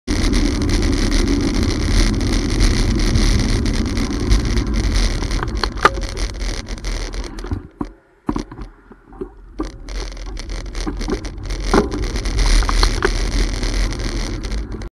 Ealing Road, South Ealing, 22 April 2018
A cyclist prepares to turn right by signalling, moving to the right and waiting for oncoming traffic to pass. The driver of a 4x4 behind the cyclist decides to close pass the cyclist, risking pushing the cyclist into the path of oncoming traffic for the sake of saving four seconds.